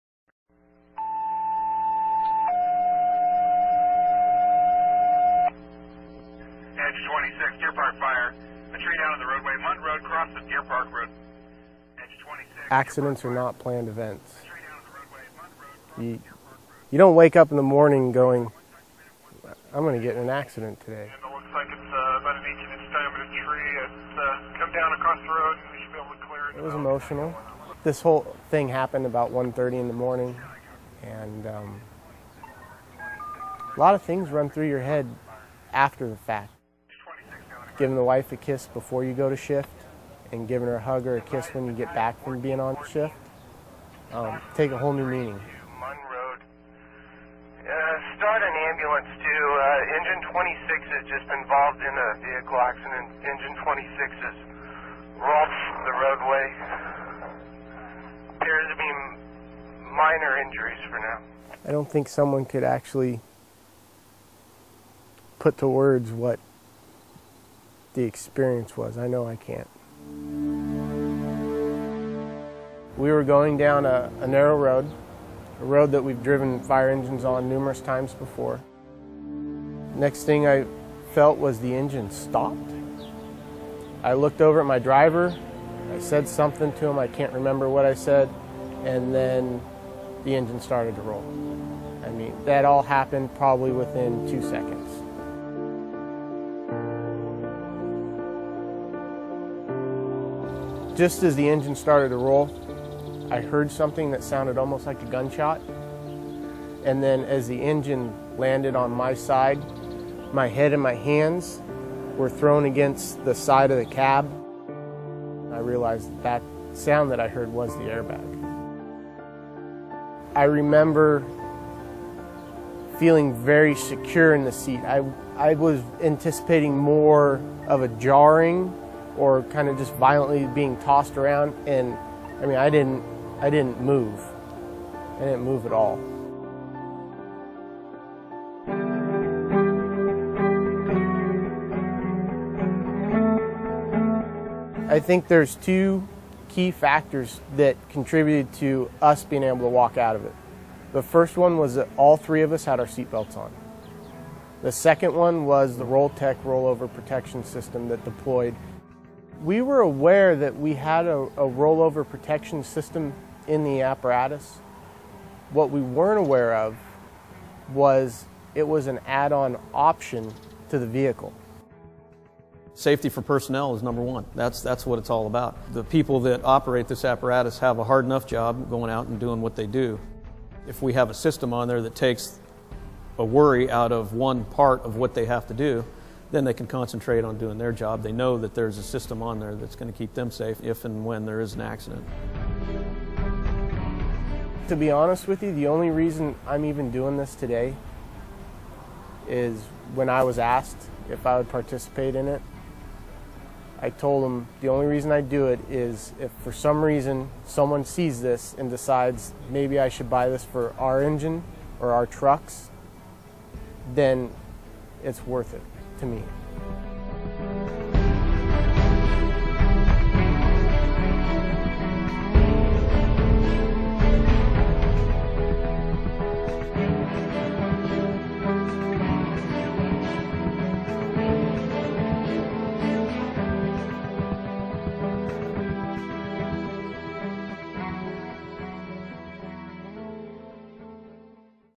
Firefighters in Rollover Crash
A fire truck veers off the road in a violent roll over accident, while en route to an emergency. Fortunately, this fire fighter and two other passengers were safe inside the fire engine. The airbag system--RollTek--successfully deployed to protect the head from colliding with the side of the vehicle, as the fire engine flipped 360 degrees down a mountain. As a result, all three fire personnel remained alive.